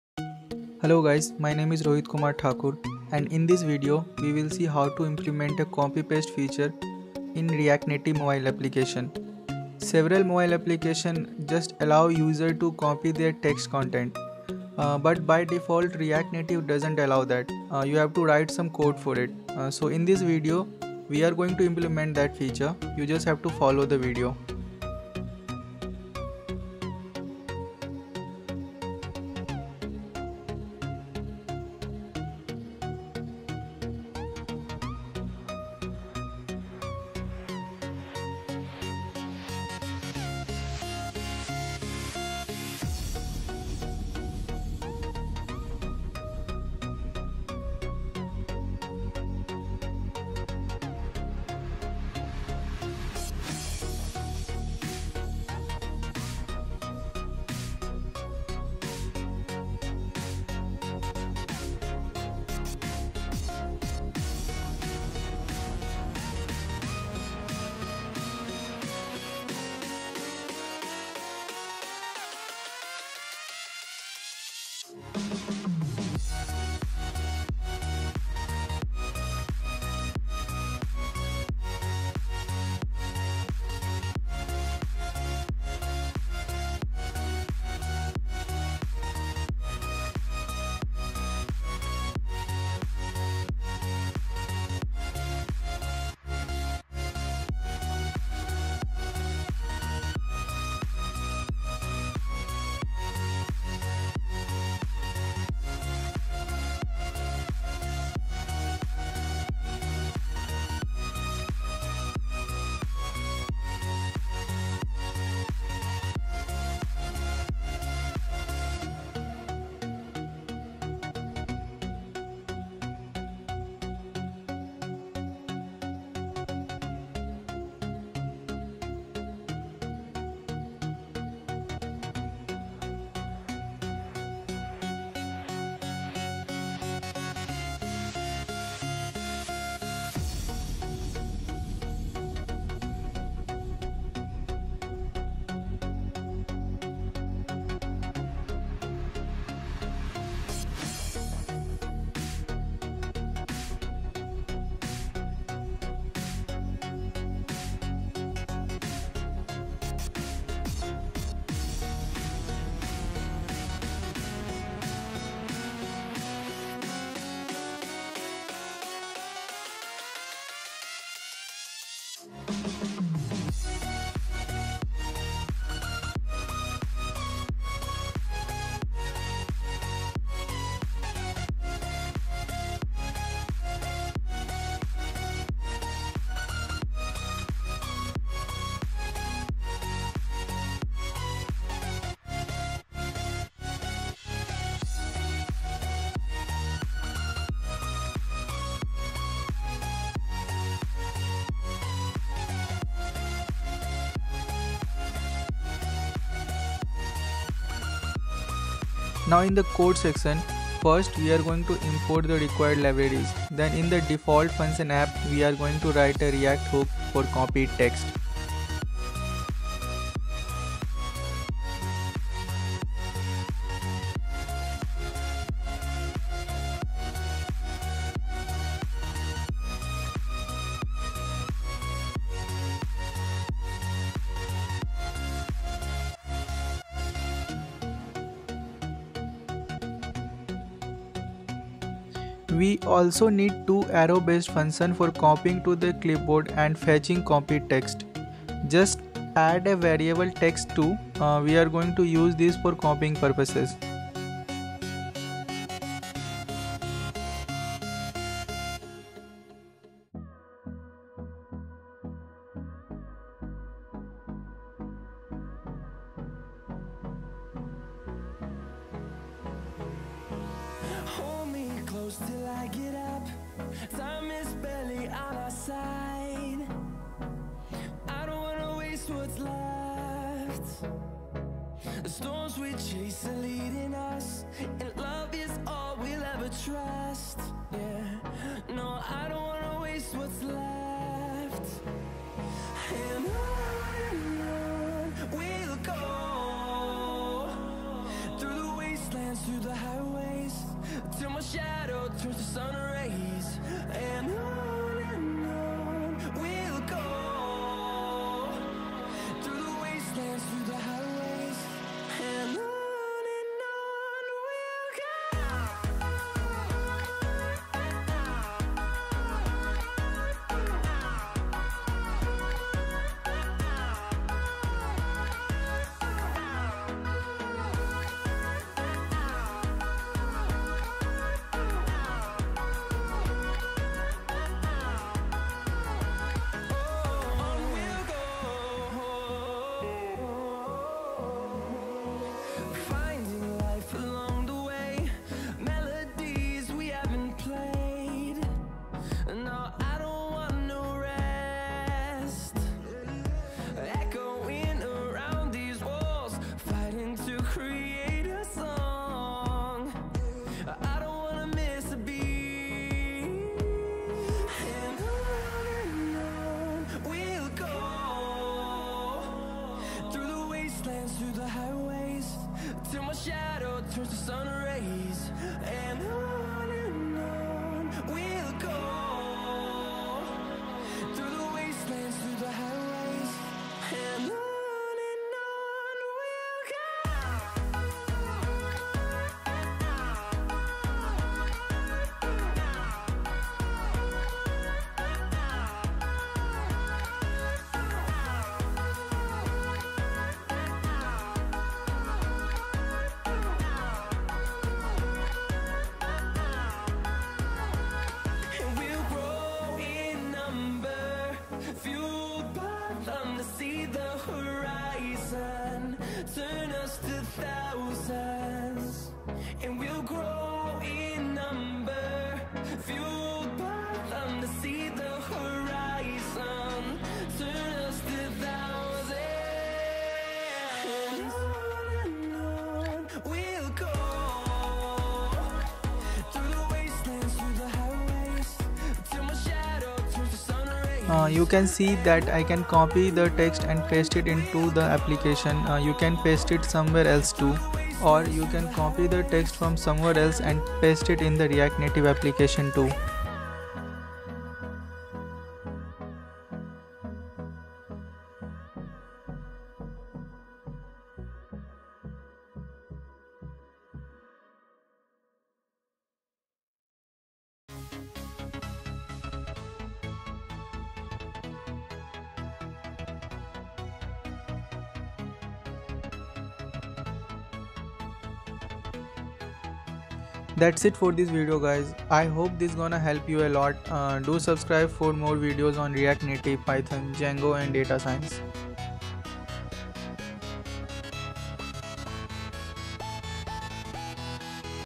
Add Copy Paste Feature In Your React Native Expo Mobile Application || Android & iOS || JavaScript
In this video we are going to add a copy paste feature in our react native mobile application. You can copy text from anywhere and paste it into your react native expo mobile application. Or just select the text of react native expo mobile application and paste it somewhere else.